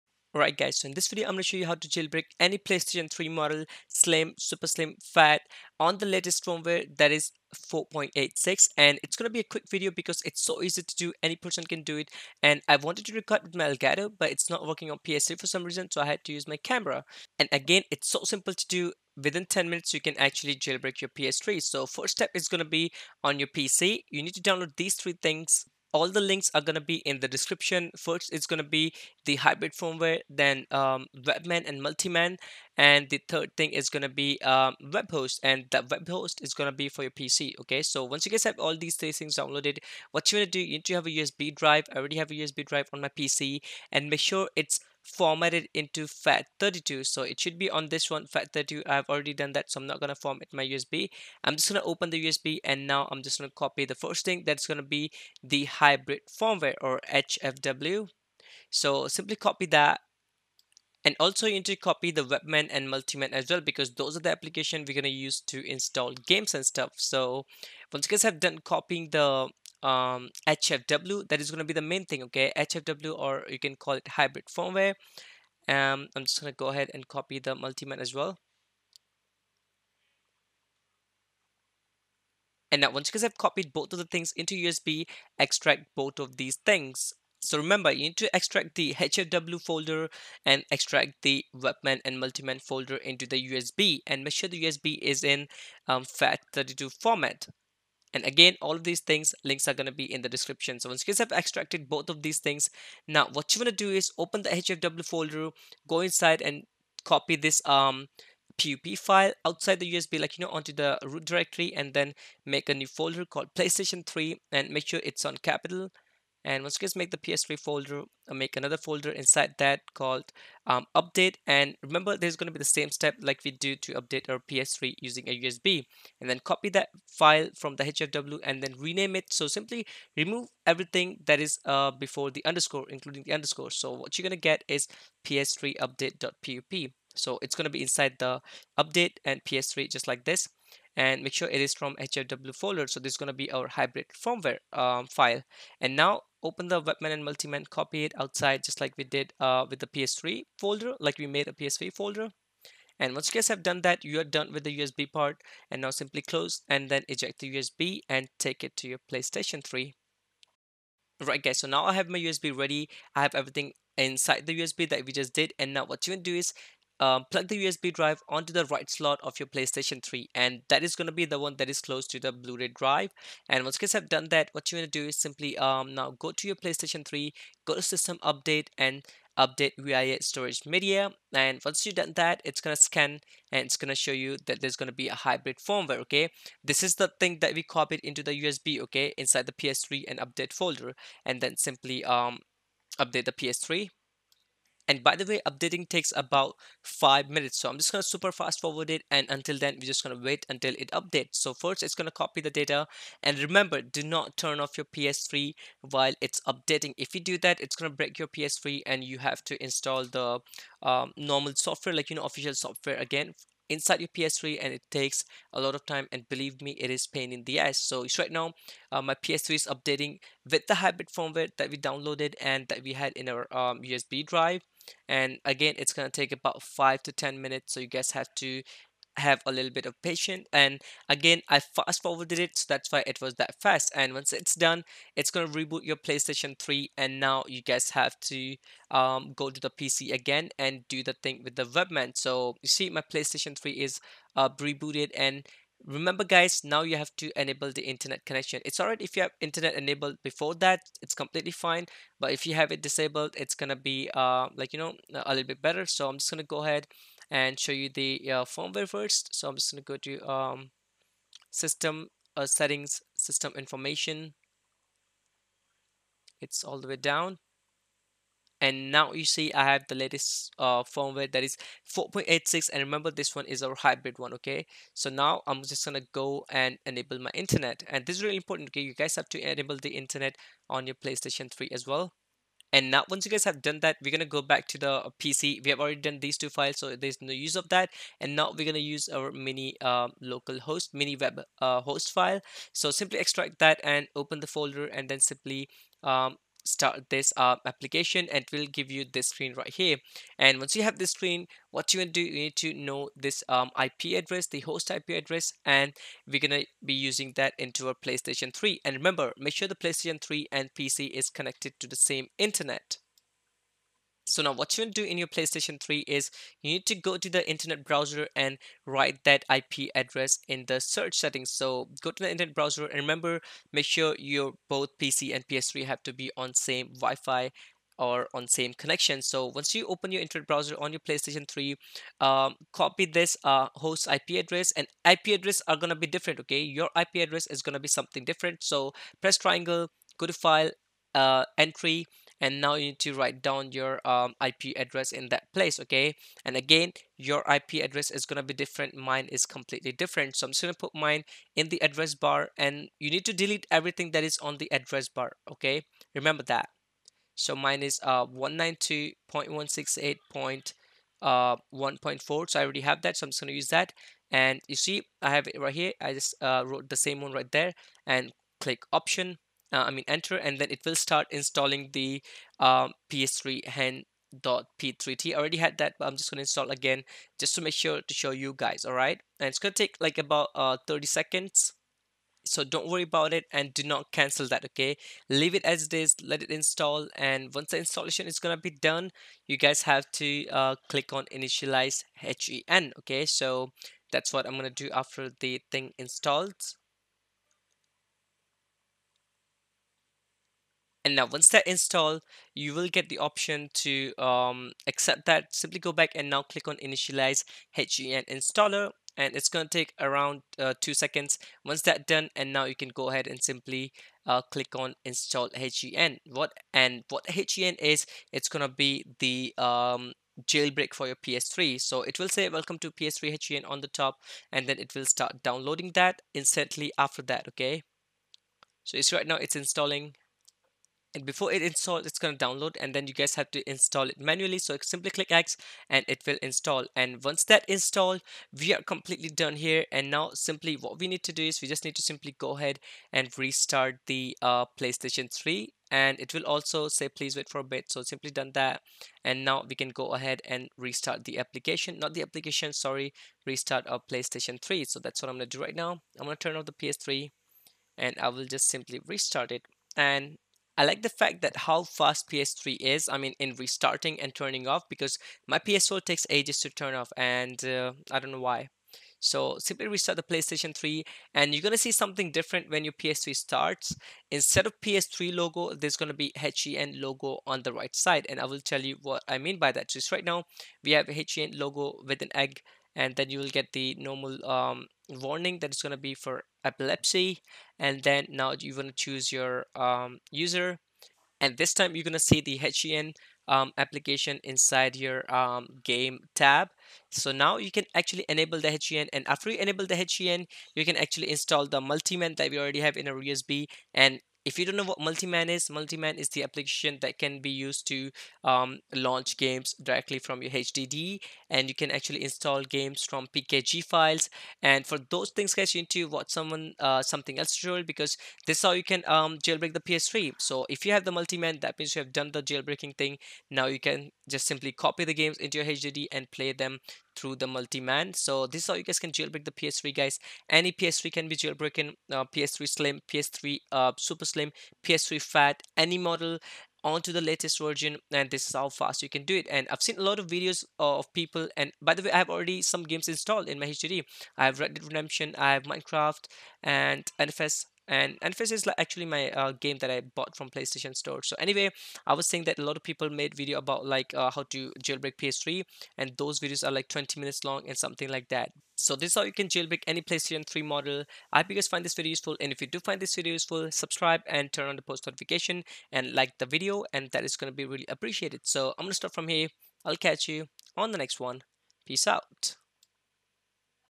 How To JAILBREAK Any PS3 Model In 10 Mins! 4.86 (Ps3 slim,Super Slim,FAT) in 2020
*ANY PS3 CAN BE JAILBROKEN*

In this video i am going to show you how to jailbreak any ps3 model withing 10 minutes easily and anyone can jailbreak their ps3 by this tutorial.